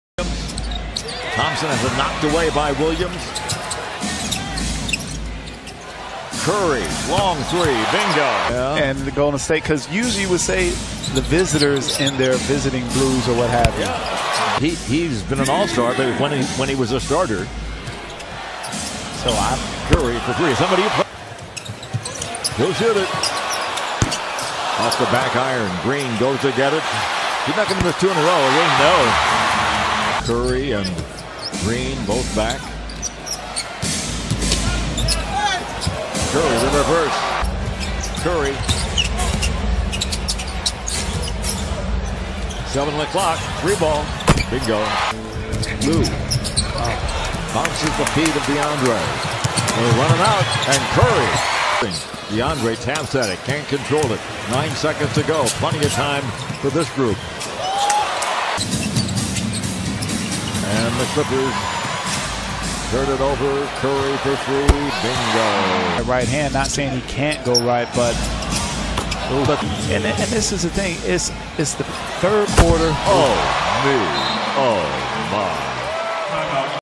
Stephen Curry'den sadece 30 dakikada 8 üçlük, 45 sayı
Warriors - Clippers 6.1.2018